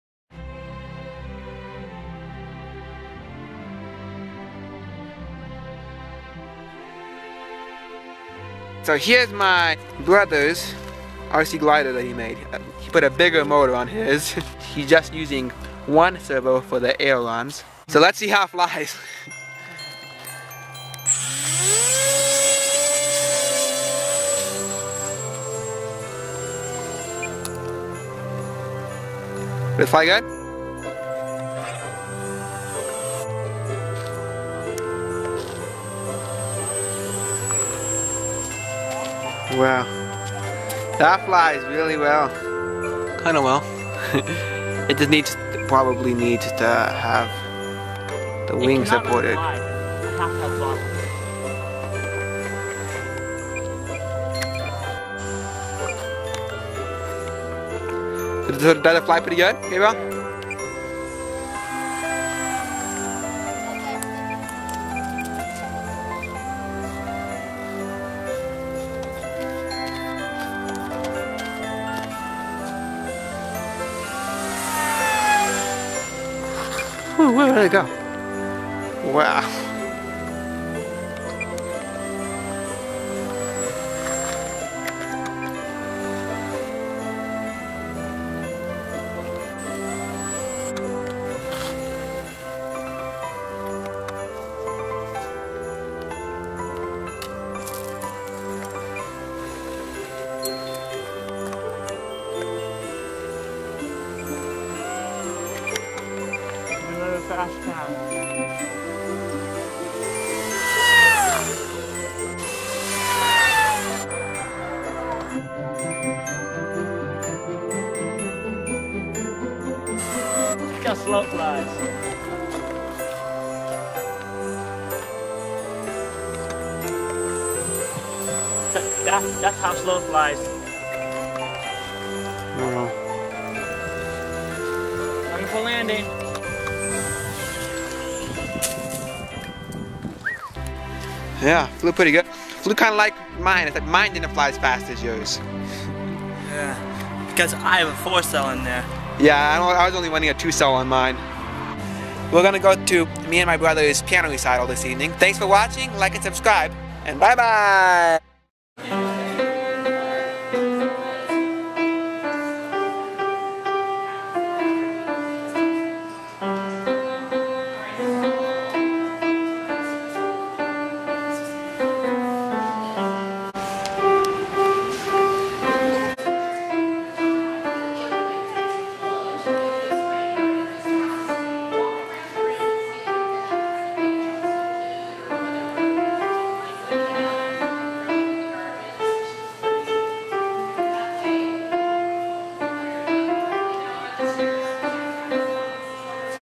Fast 4s RC Glider//Test Fly RC
My brother made a glider RC!    He used a 4s to fly this glider, it flew really fast!